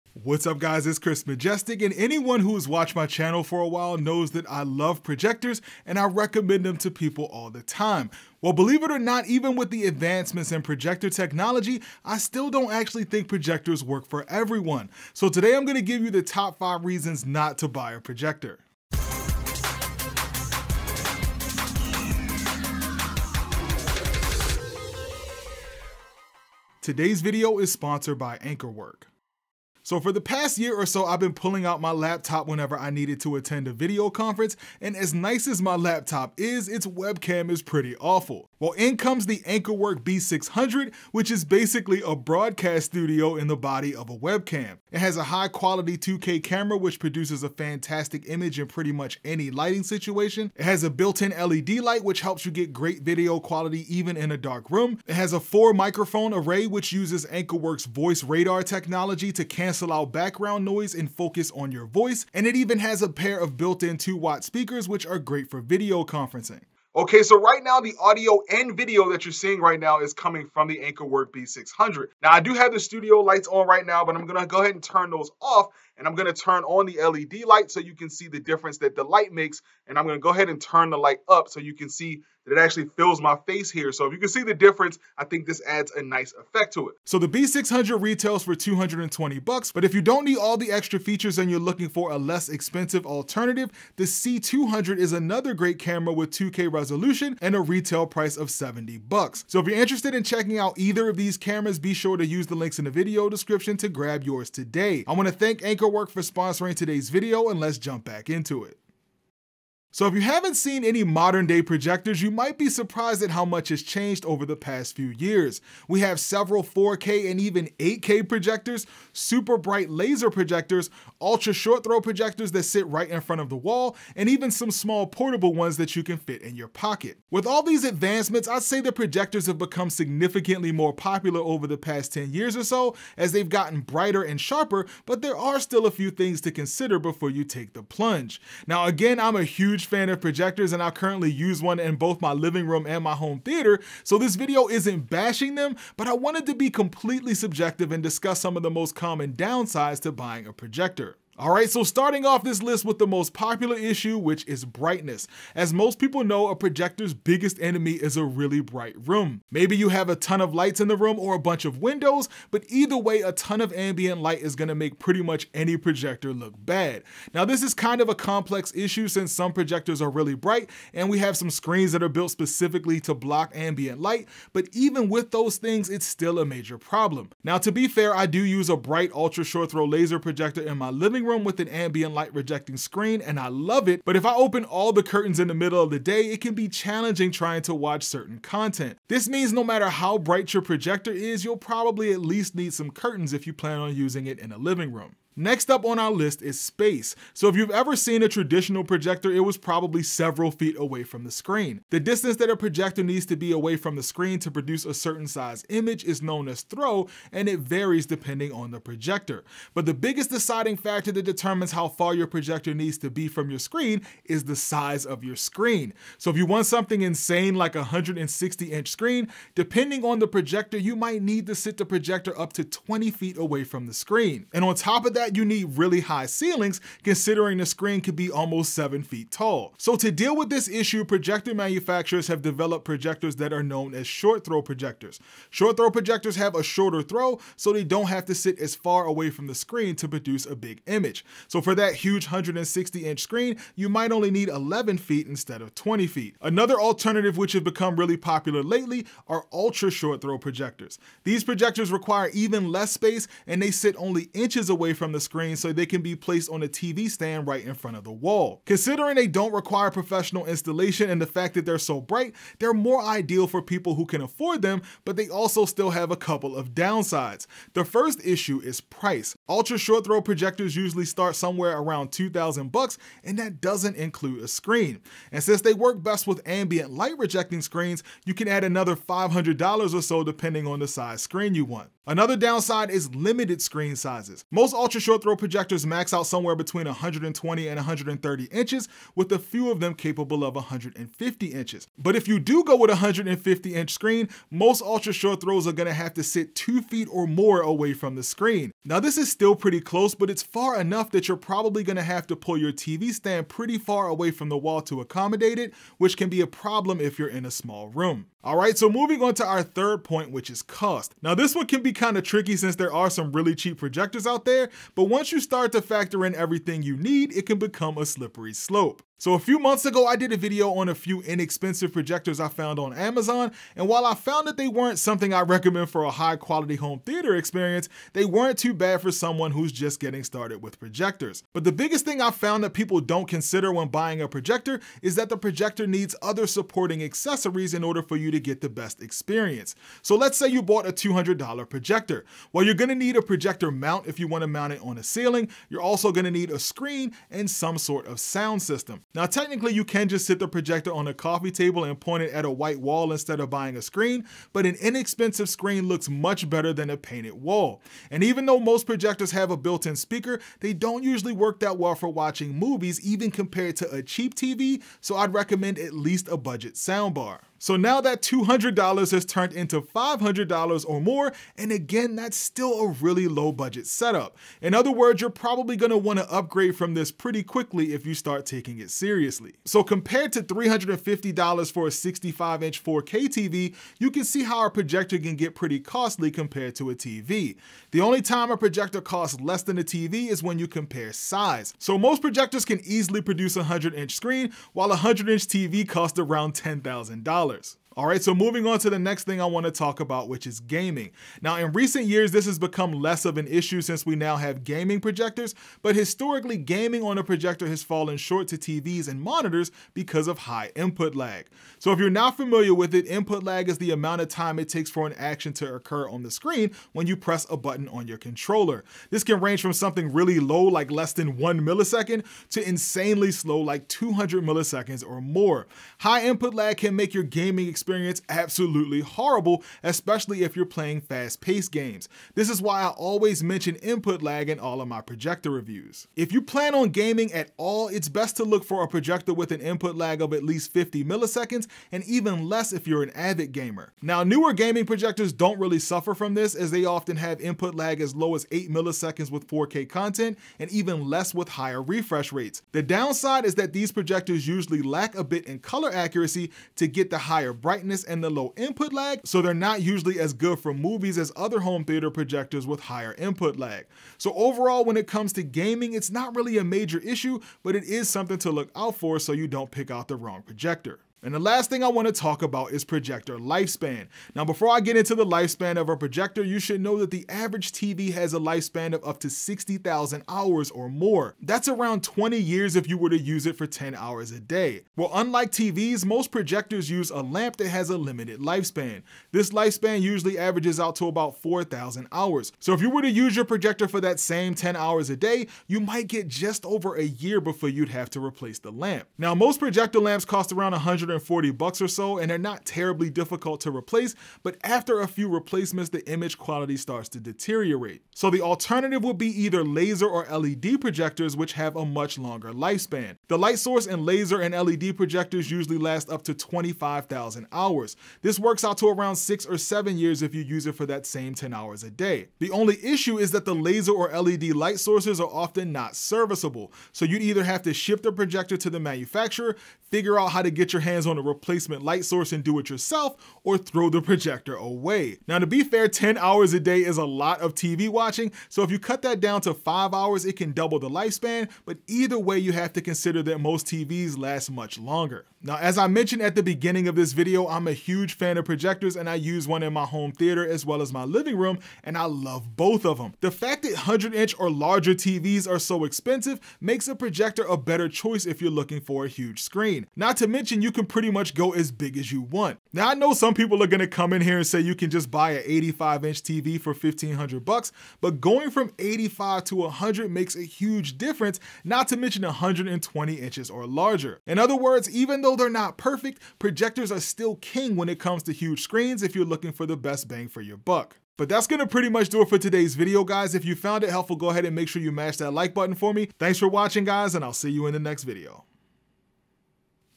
5 Reasons NOT To Buy A Projector - The Real Truth
Being a big fan of modern projectors I regularly recommend projectors to people but believe it or not, I don't believe projectors work for every situation. Here are 5 reasons to stay away from a projector.